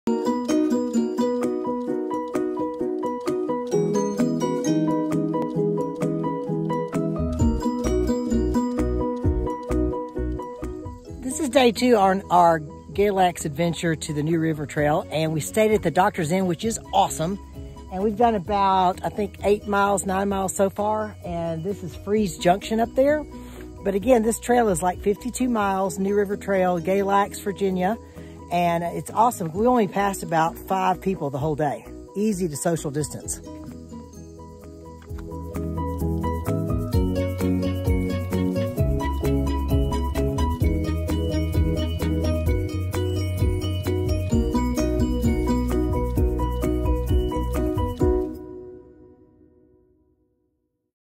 Galax To Fries beautiful ride
Biked from galax where we stayed at The Doctors Inn (love it) Then drove half mile to start of New River Trail On that end. Rode 12
Miles to junction to take trail to Fries Va. had a quick bite at the cafe next to the trail and headed back.  37 miles total